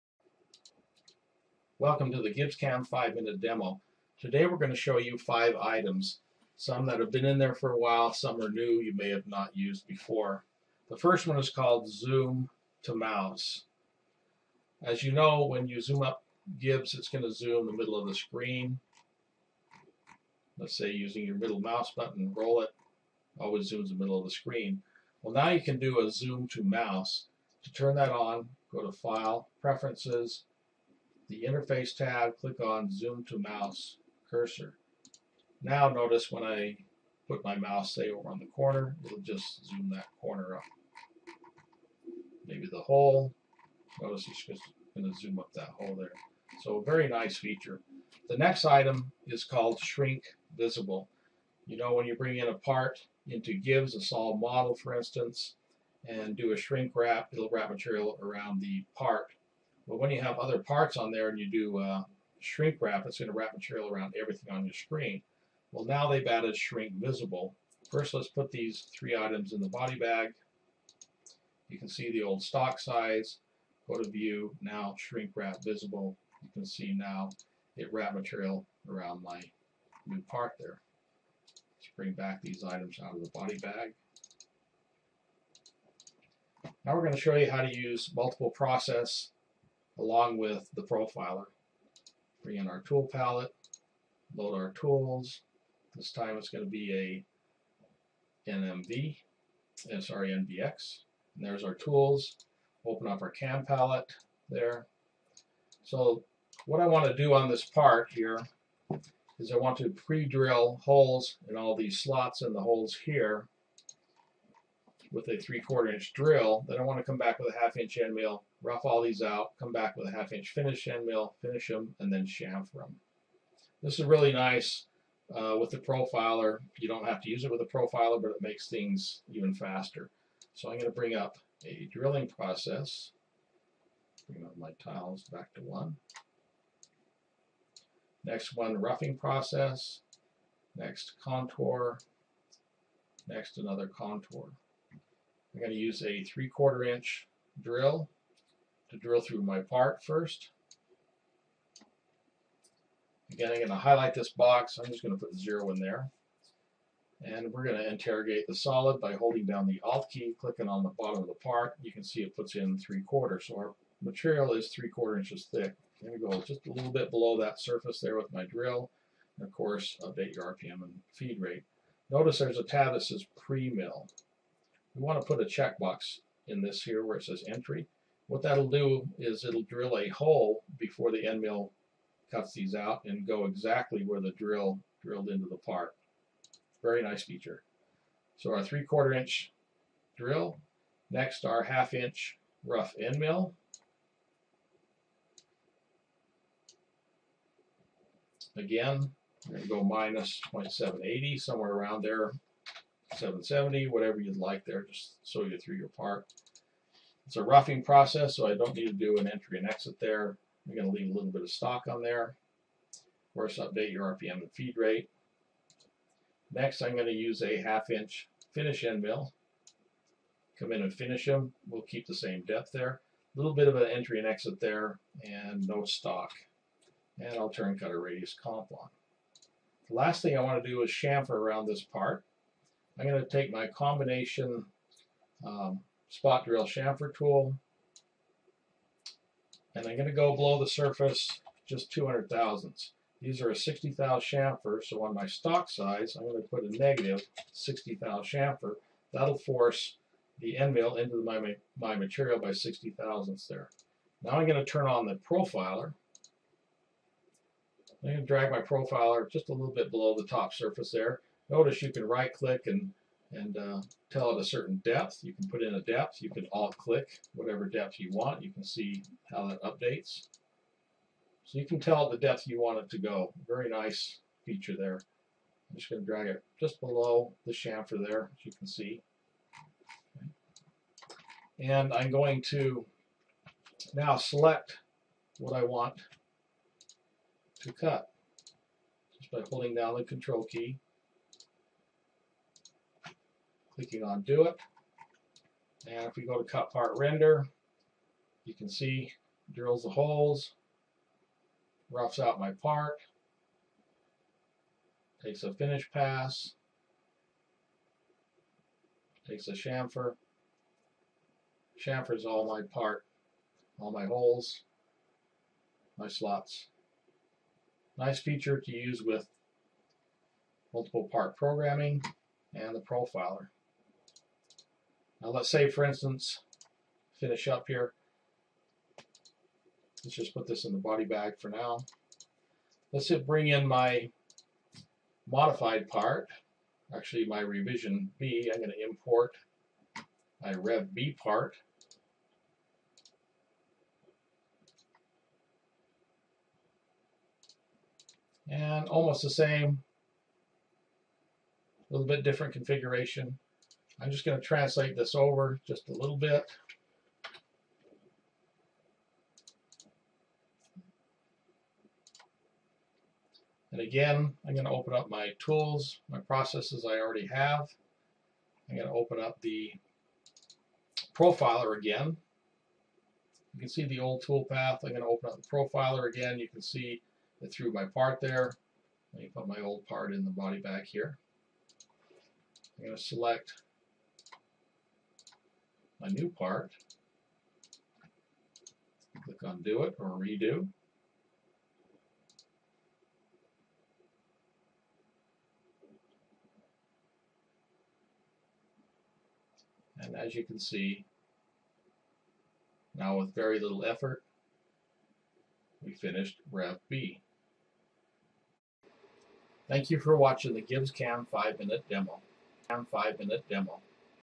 GibbsCAM, multi-process programming
GibbsCAM, learn how to use multi-processes as well as other tips